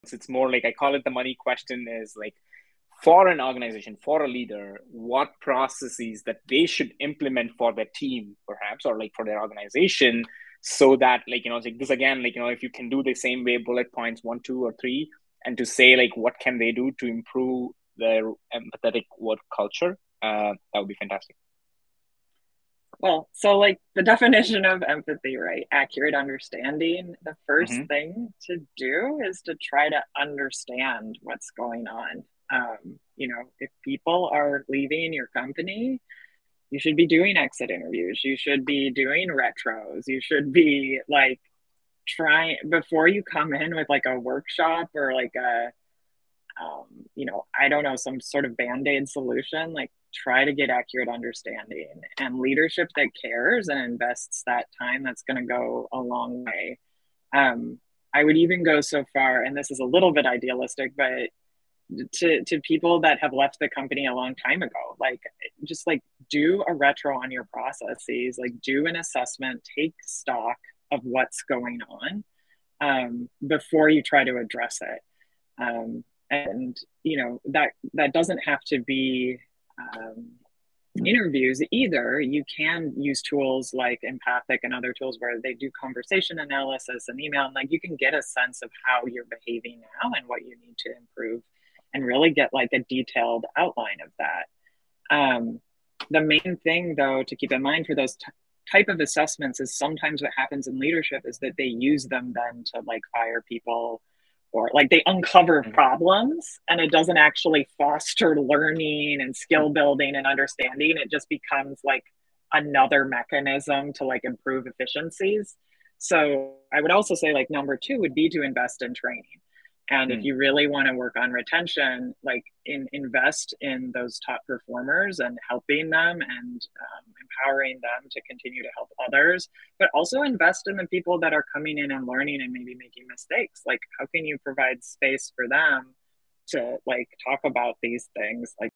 Episode #10 Extract 1 -What processes should they implement for an Empathetic Work culture?
For an Organization for a leader. What processes should they implement for an Empathetic Work culture?

Grin will start with the definition of Empathy “Accurate understanding of emotions and feelings of another”, so the first thing to do is understand what is going on, if people are leaving do exit interviews, retros to understand why? Leadership that cares and invests that time is going to go a long way.

1. Do interviews and retros uses systems to understand why people are leaving.
2. Invest in training, and then invest in the top performers to retain them.